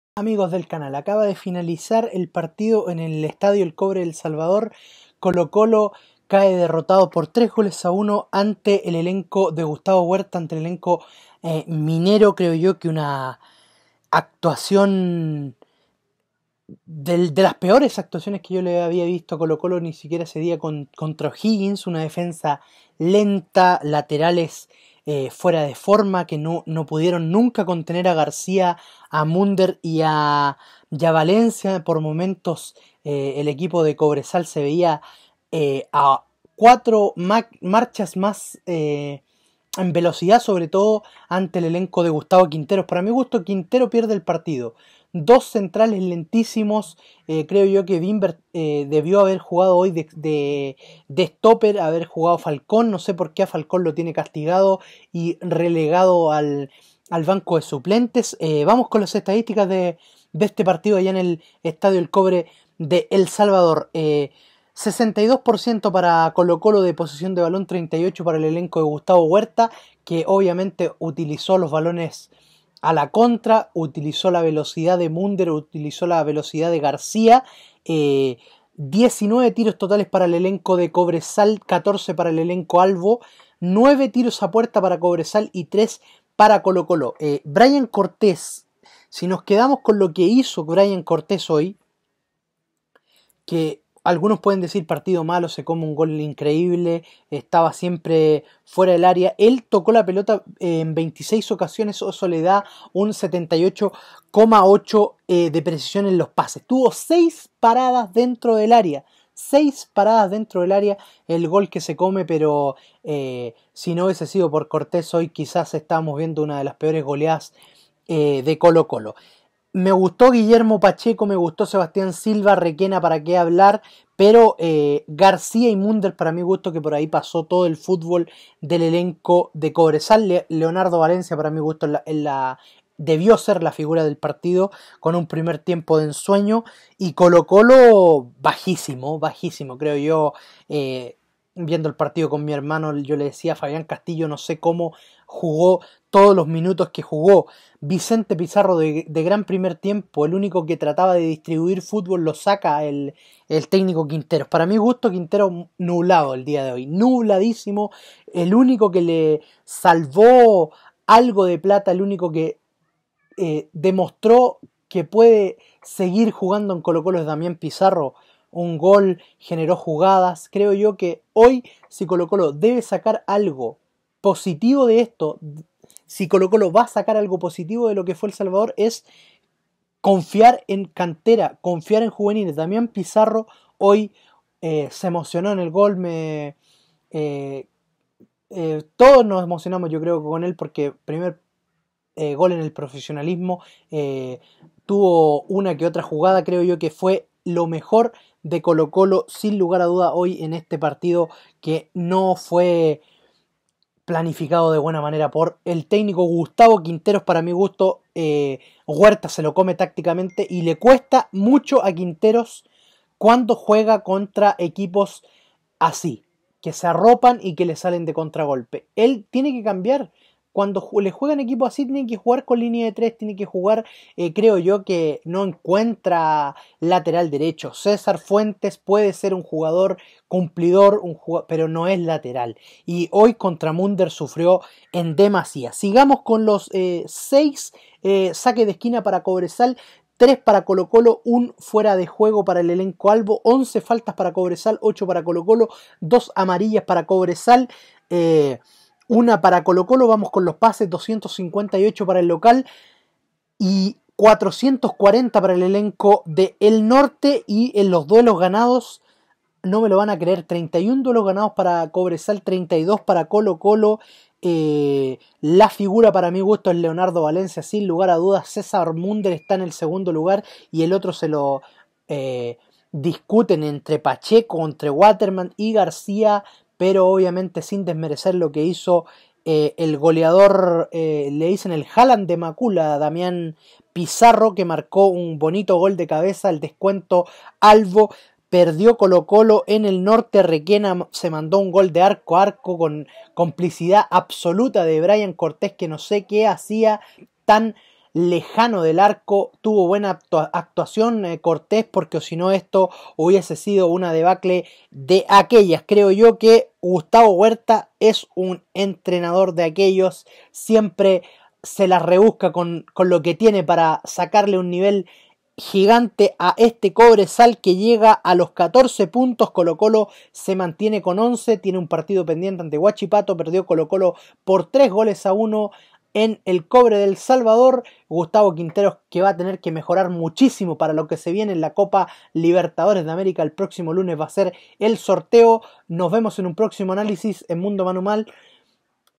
COBRESAL 3 1
COLO COLO PIERDE UN PARTIDO DE AQUELLOS ANTE UN COBRESAL DE GUSTAVO HUERTA QUE SE COME TACTICAMENTE DE CONTRA AL ELENCO DE QUINTEROS QUE PRESENTO UNA DEFENSA LENTA EN LA ALTURA DEL SALVADOR Y UN BRAYAN CORTES QUE PRESENTO LAMETABLEMENTE LAS DOS CARAS DE UN PORTERO

EL SHOW DEL ANALISIS DEL FUTBOL

    ^ MUNDOMANUMAN ^